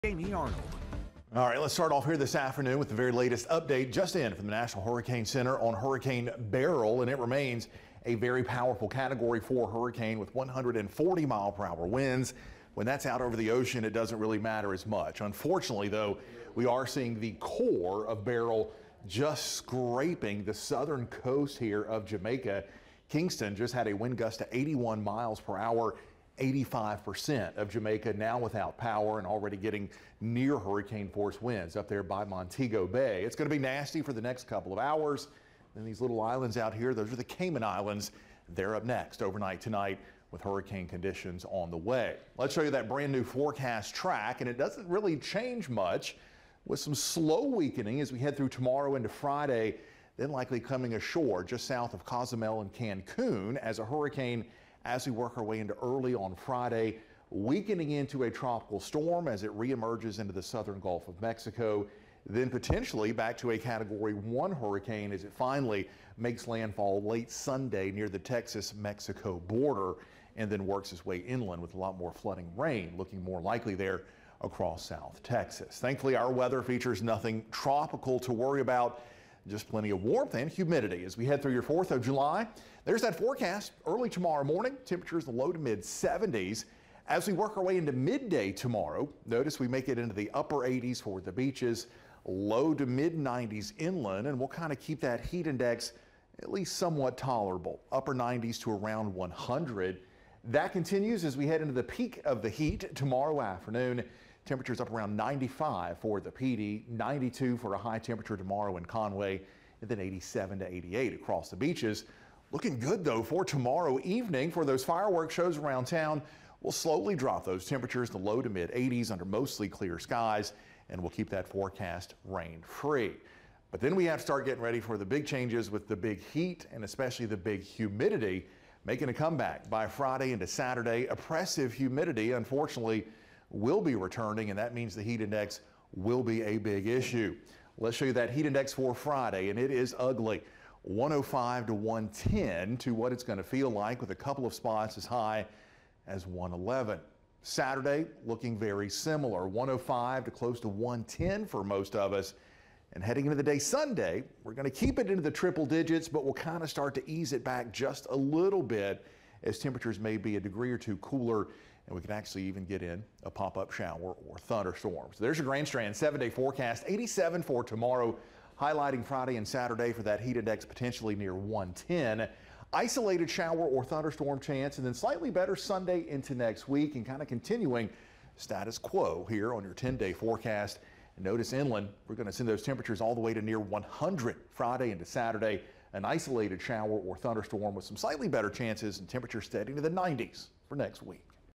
FIRST ALERT: Hurricane Beryl remains extremely powerful
Beryl continues to move along the southern coast of Jamaica as a category 4 hurricane.